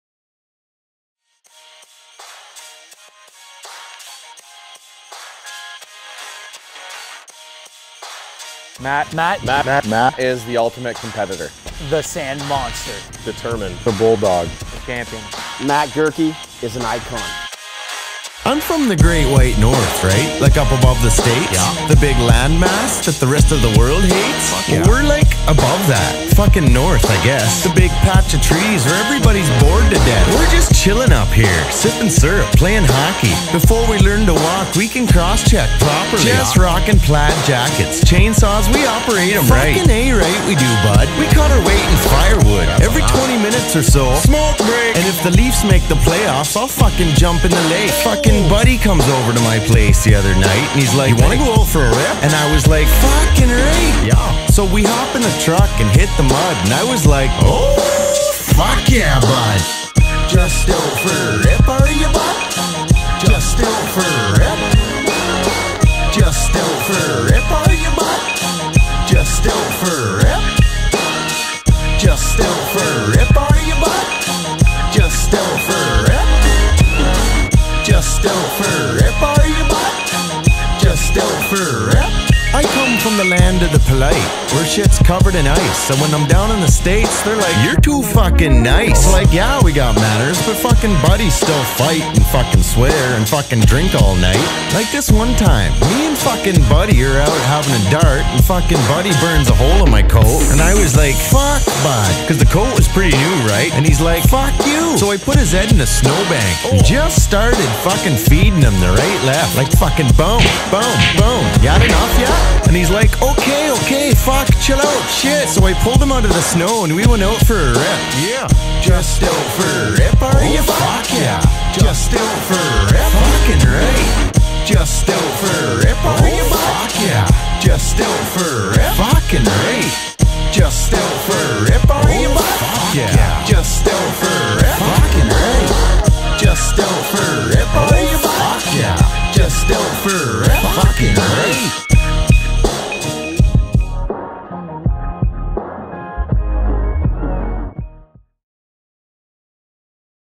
Matt Goerke Supercross Rip / Motocross Editor
I edit motocross videos like this one with Canadian champion Matt Goerke ripping up a supercross track.

...Free Moving Quote...